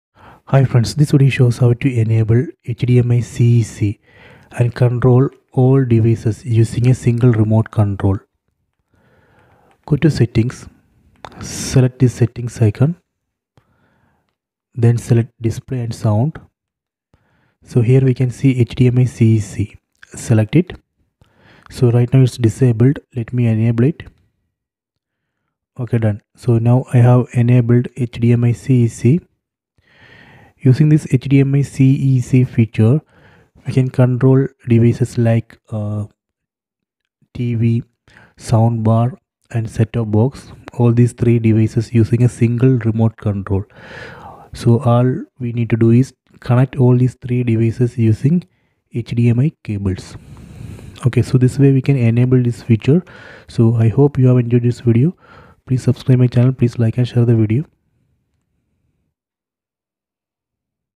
HISENSE Smart Google TV : How to Enable HDMI CEC Device Control | TV remote to Control Other Devices
Google TV,
How to Google TV,
Android TV,
Smart TV,
Android TV OS,
How to,
android,